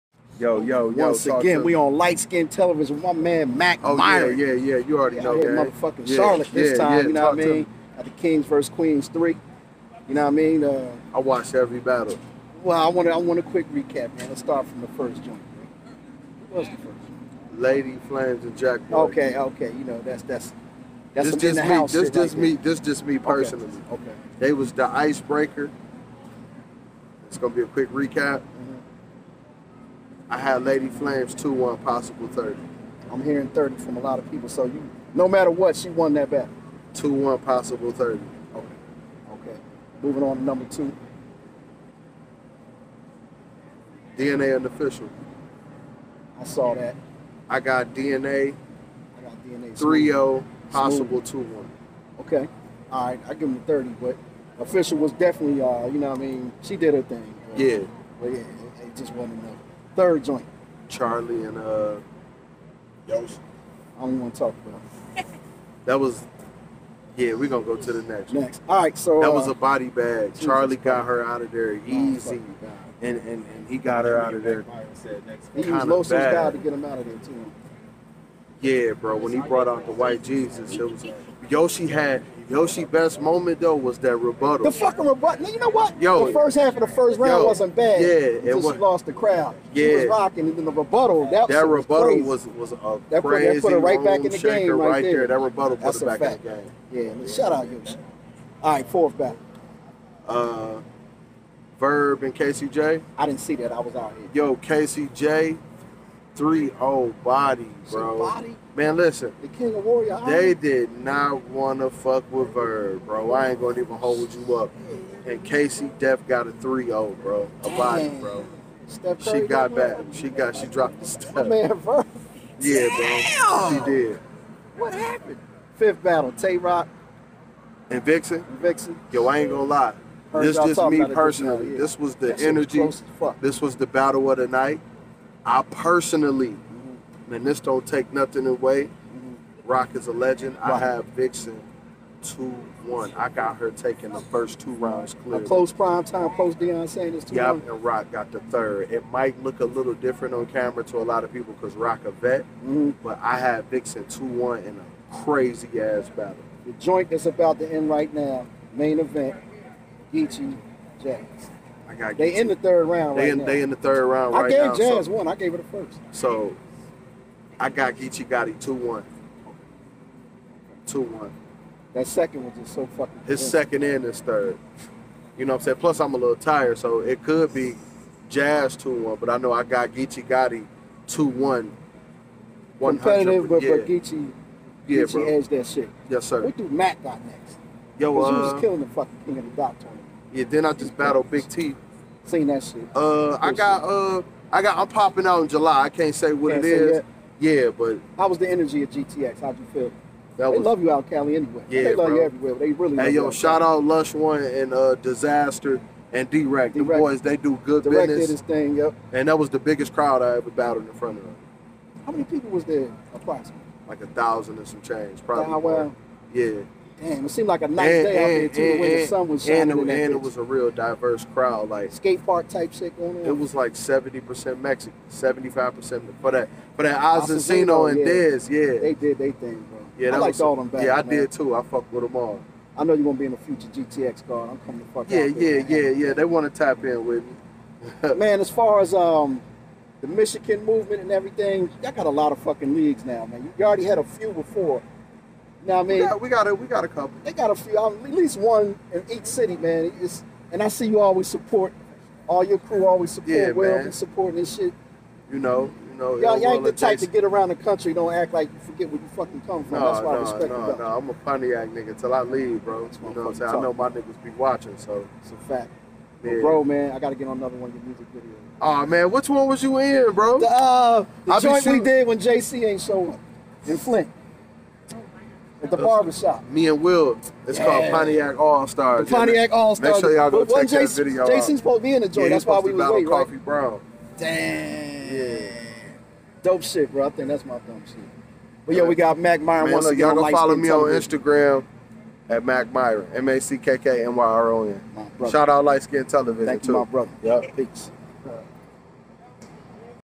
Mackk Myron interview at SMACK URL- Kings vs Queens 3, giving LIGHTSKIN YU a main event day RECAP !
Light & Mackk chop it up as usual.